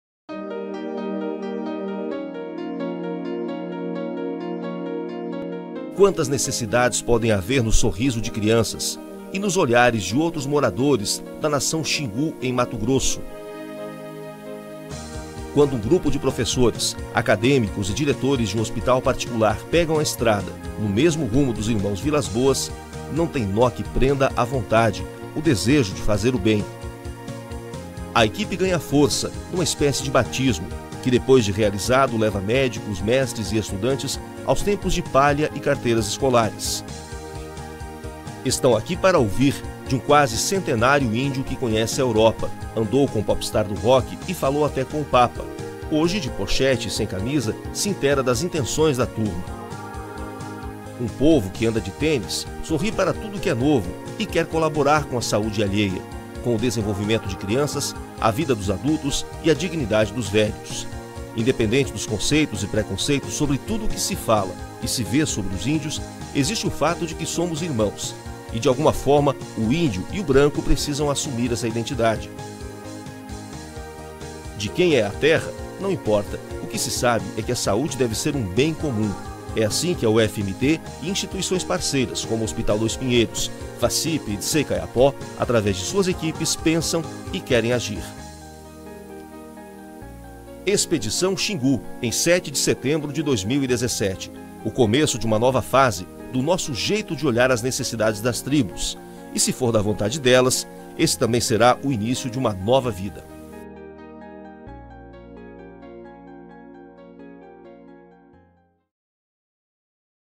XINGU LEGENDADO
Xingu Project (Xingu Expedition) 2017 - The Xingu Project is a project we started in 2017, and this year was our second year with this work. We went to the Xingu Indigenous Park. Xingu is the home of 16 different indigenous ethnicities. In 1750 there were a dozen of million of natives living there. At the end of the nineteenth century, they were 3000, most of them killed or captured to slavery or “white men diseases”. And now a days they still face many big problems, and two of them is alcohol abuse and health assistance. In September of 2018 We went to Xingu in three physicians (all professors of the School of Medicine from the Federal University of Mato Grosso), one dentistry, 10 med students, one dentistry student and 4 volunteers. This year we visited three villages, we did 250 medical consults, donated 49 eye glasses, we found 14 natives with cataract (10 of them will be submitted to cataract surgery in November), and 61 dental procedure. We traveled 550 road and terrain miles (more than 18 hours), 5 hours in boats. Our Med students performed a theater play where they show how the alcohol abuse destroys their lives, families and culture. Also they presented a talk where they discussed the many problems and diseases that are caused by alcohol abuse. Meetings with native women and kids were also done to teach them about health, healthy food and kids dental care (many natives loose teeth before adult age).